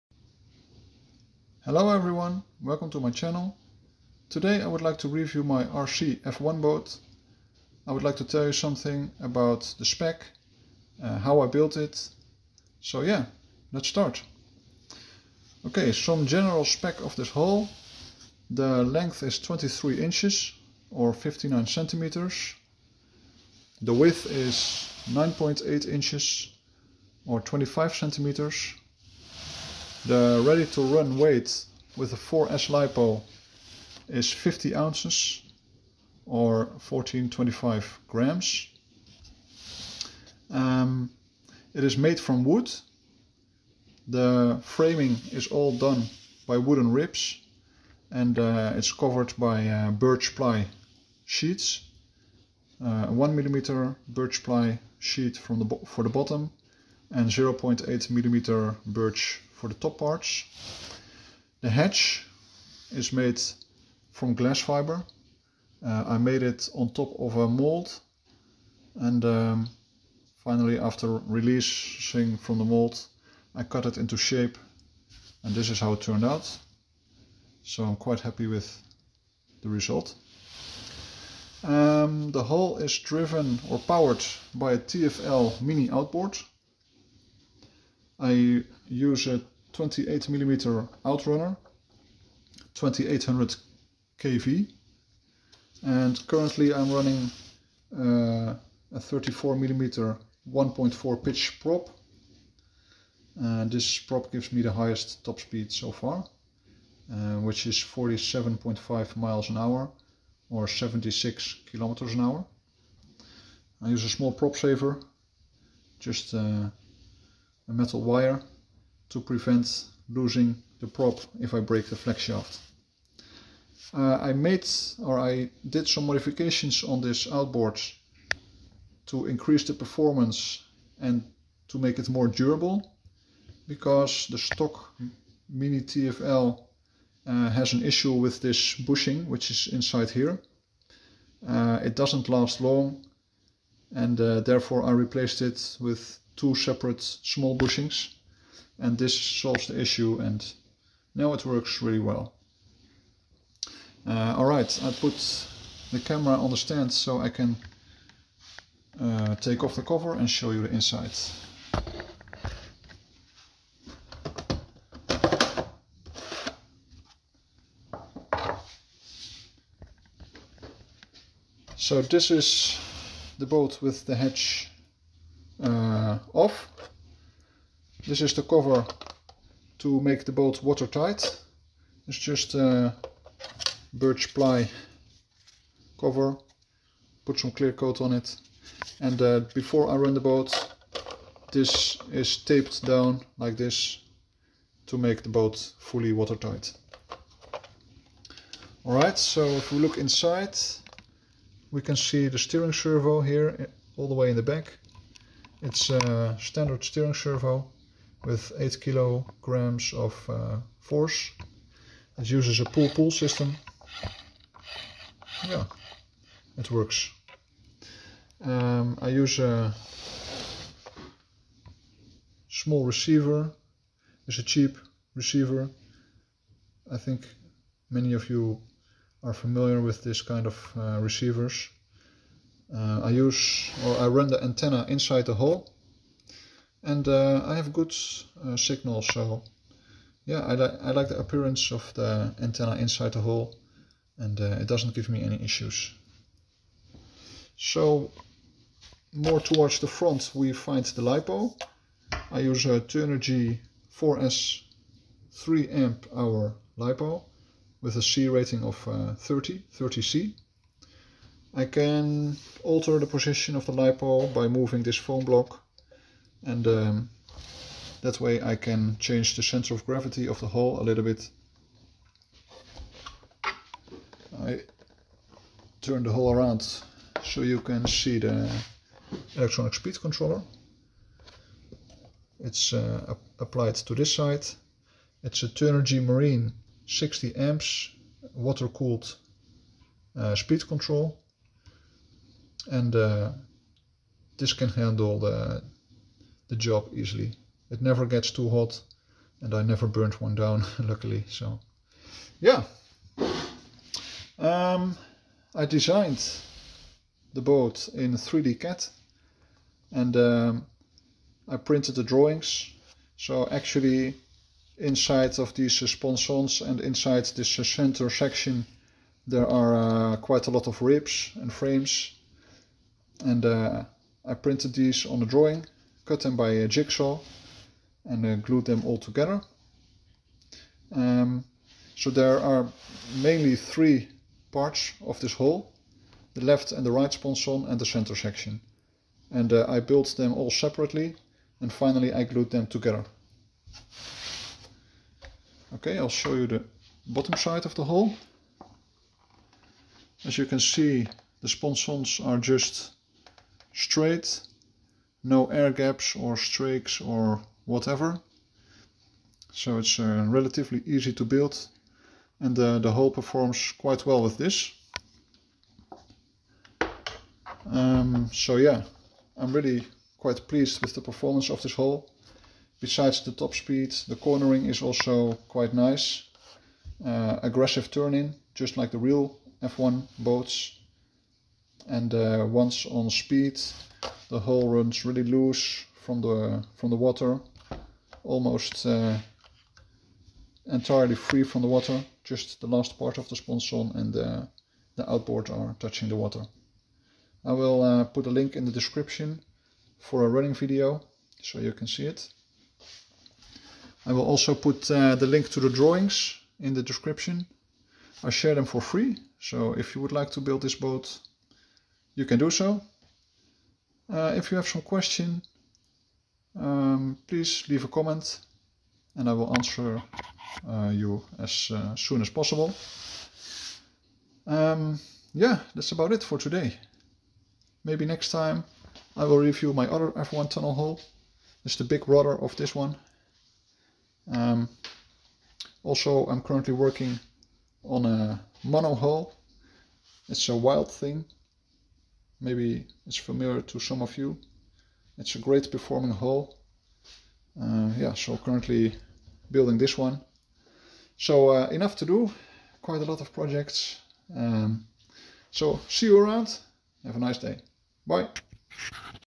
DIY rc boat: Cozy F1 Tunnel powerboat review
This is a review of my DIY rc F1 Tunnel boat. I tell about the specifications, the backgrounds of the build and I explain the setup.

Build one yourself? Free drawings can be found here (at the bottom of the page):